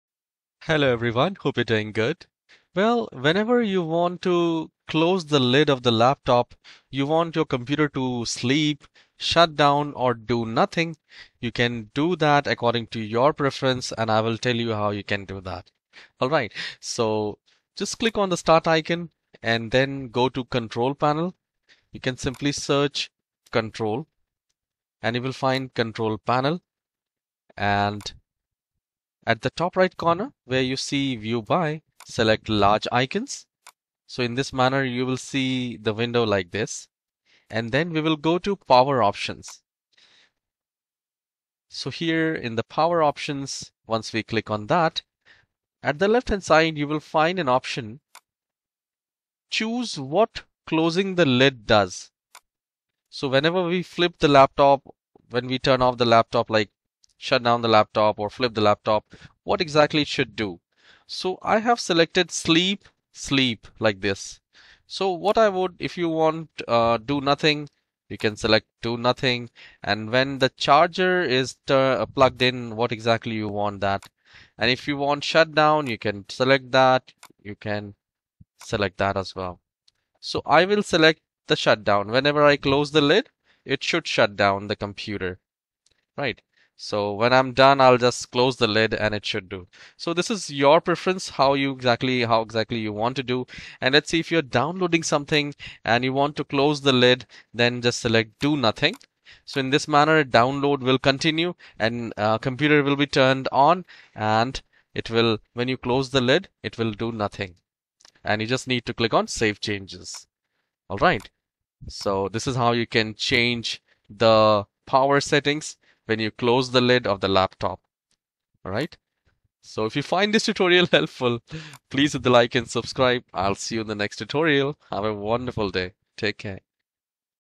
Customize What Happens When You Close Your Laptop Lid in Windows 11
Want to learn how to customize the laptop close lid settings in Windows 11? This video guides you through adjusting these settings to control what happens when you close your laptop lid – whether it’s to put your device to sleep, keep it running, or shut it down. These Windows 11 lid settings allow for greater convenience and can be customized to meet your unique needs, whether you're on the go or working from home. Follow our step-by-step instructions to ensure your Windows 11 laptop lid settings are optimized for your preferences.

In this tutorial, we cover:

How to change what happens when you close the laptop lid in Windows 11
Customizing power and sleep settings for laptops in Windows 11
How to keep your laptop running even with the lid closed
Troubleshooting common issues with laptop close lid settings
Watch the video and get the most out of your Windows 11 laptop with these quick settings tips!